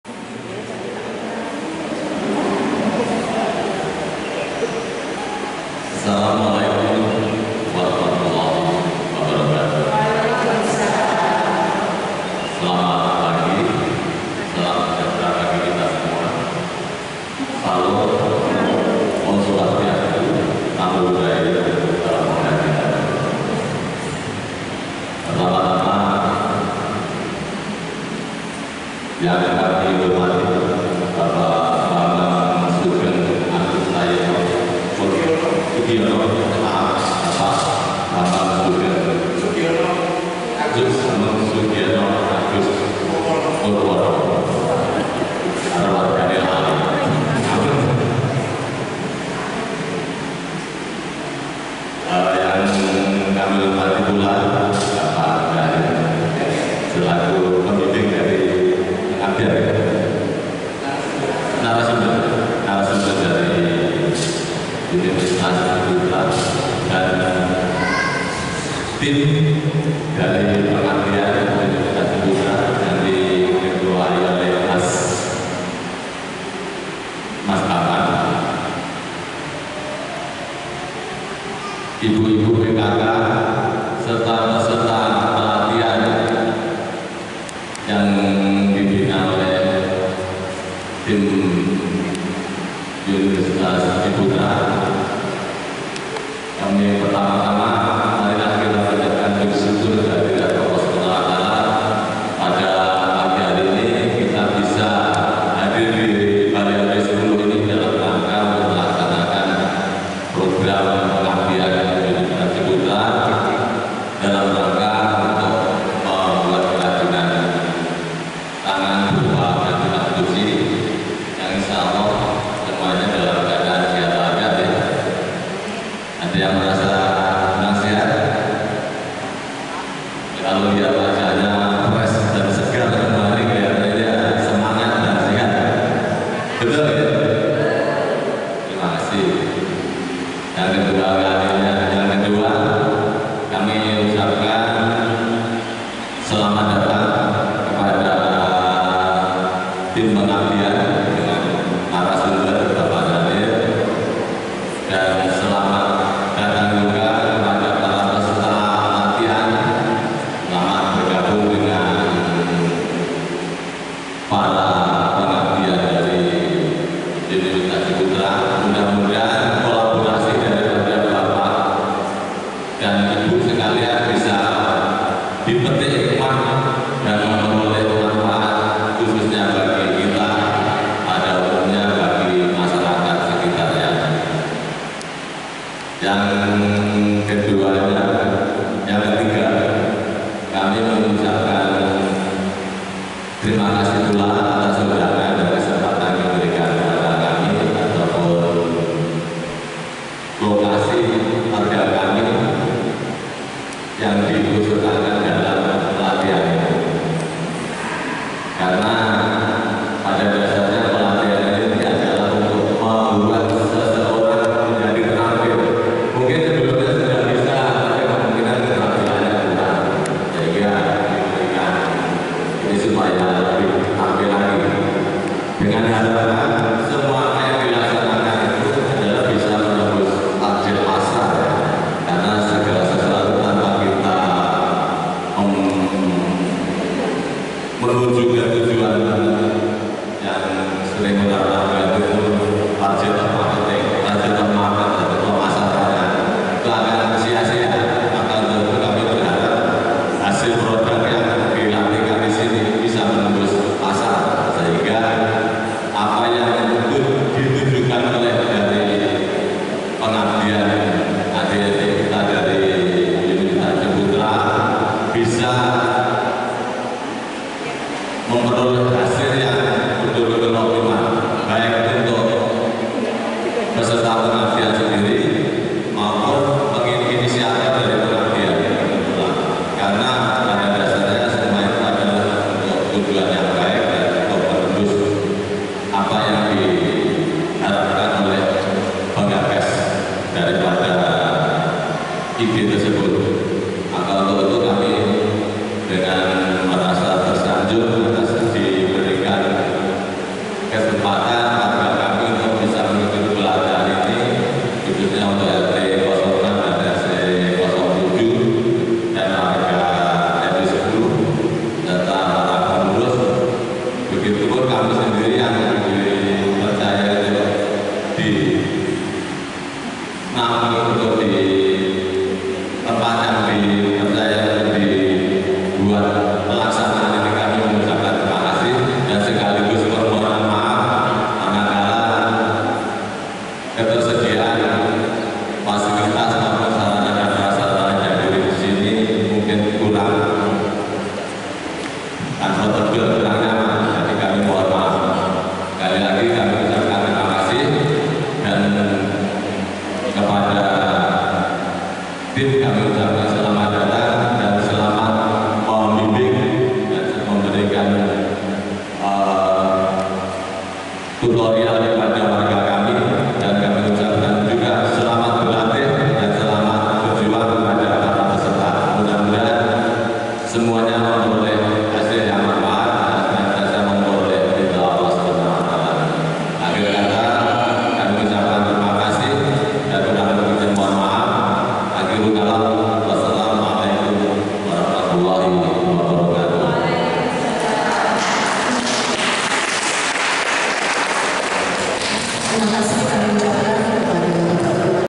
DIMAS UC Sambutan Ketua RW 10
Acara Pengabdian kepada Masyarakat Universitas Ciputra Surabaya (DIMAS UC) dengan judul: Pelatihan pembuatan dan cara memasarkan gantungan kunci bagi Ibu PKK dan Pemudi di RT 6 dan 7 RW 10 Kelurahan Lesanpuro, Kecamatan Kedungkandang, Kota Malang. Pelaksanaan hari Sabtu dan Minggu (tanggal 21 dan 22 Oktober 2023), bertempat di Balai RW 10 Sabha Nusantara. Tim Pelaksana terdiri dari: Dr. Ir. Bambang Sugiyono Agus Purwono, MSc, Dr. Ir. Denny Bernardus, MM, dan Dr. dr. Salmon Charles PT Siahaan dan dibantu Muhammad Naufaluddin, Eunice Ali, Nabila Laila, Baramza Sofa, dan Aisya Amaliah - mahasiswa program studi School and Internasional of Business and Management, Fakultas Manajemen dan Bisnis,  Universitas Ciputra Surabaya. Topik: Sambutan Ketua RW 10 Bapak Sukaji Kaka. Di-upload oleh: Bambang SAP